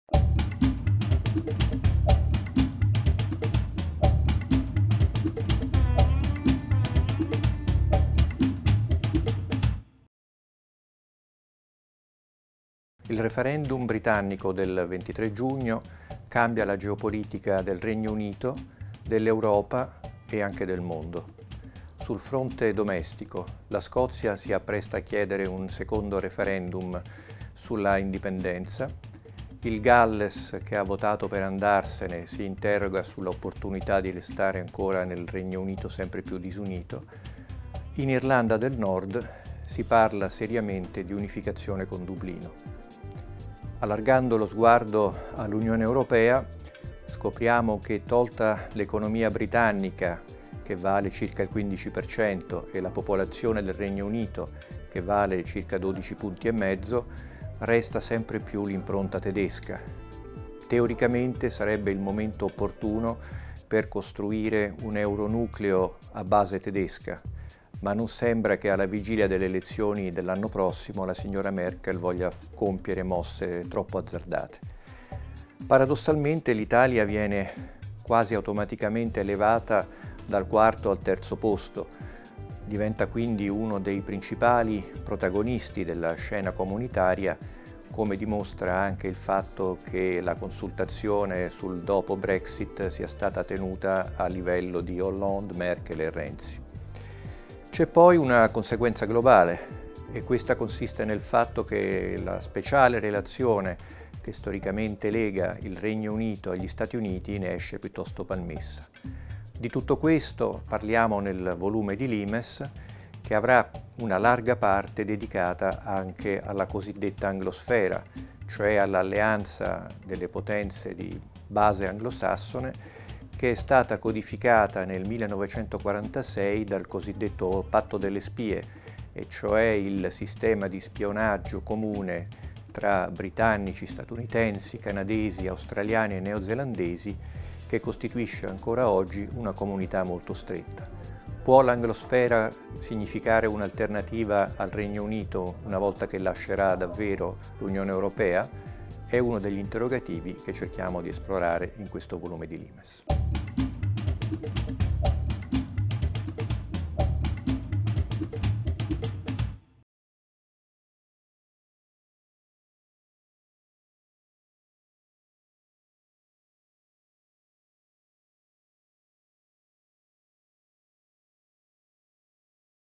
Brexit e il patto delle anglospie
Lucio Caracciolo presenta il numero 6/16 di Limes, "Brexit e il patto delle anglospie". Disponibile da venerdì 8 luglio su Limesonline, in edicola, libreria, ebook e su iPad.